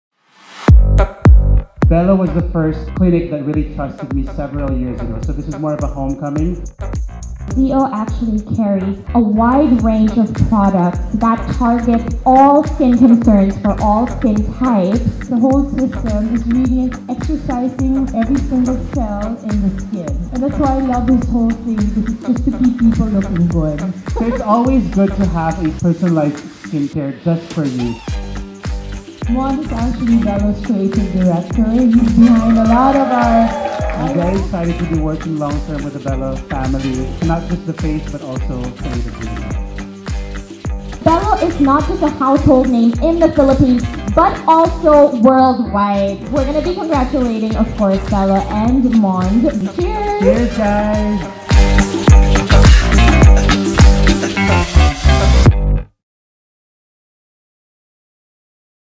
Belo x Mond Launch Party | Belo Medical Group
You just had to be there ✨ some snippets from last weekend’s launch of the ShowYourGlow campaign, celebrating Mond—the newest face of ZO® and our Creative Director! Watch to see what you missed…

Know more about Belo treatments by following us below. 👇🏼